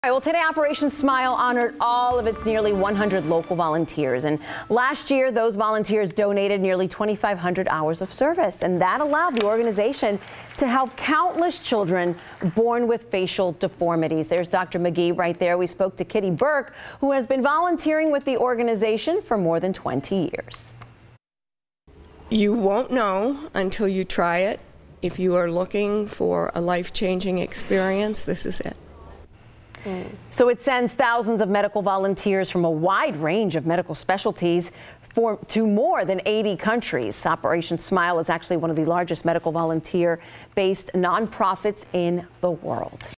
Operation Smile honors local volunteers
Operation Smile is one of the largest medical volunteer-based nonprofits.